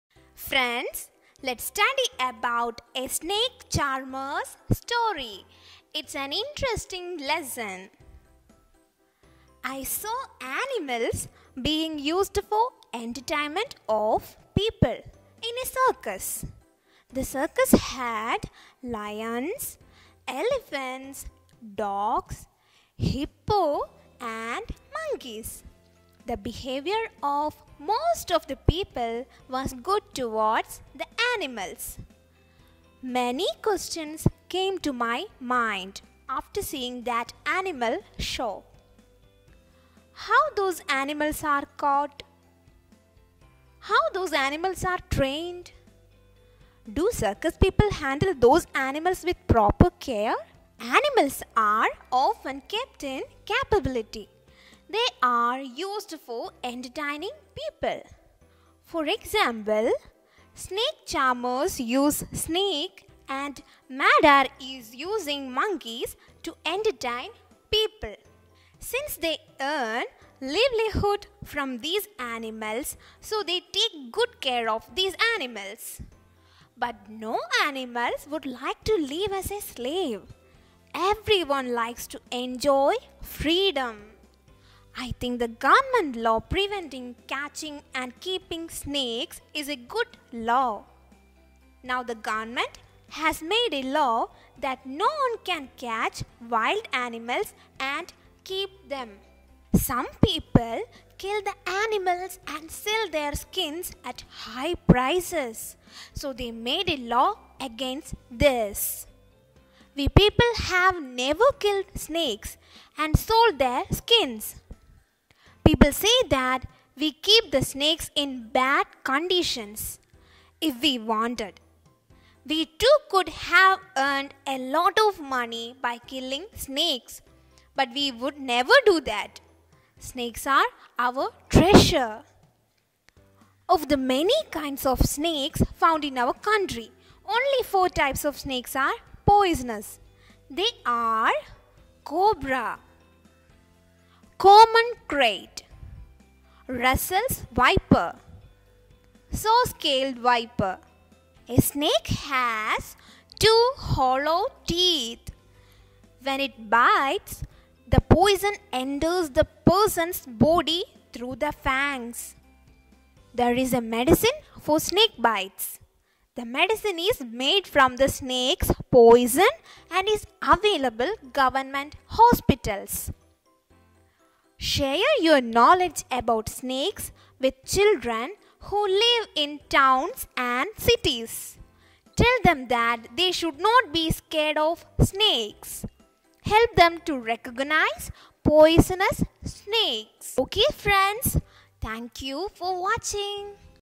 A SNAKE CHARMER'S STORY by CODES BRAIN
EVS 5th STD Chapter 2 "A SNAKE CHARMERS STORY" for CBSE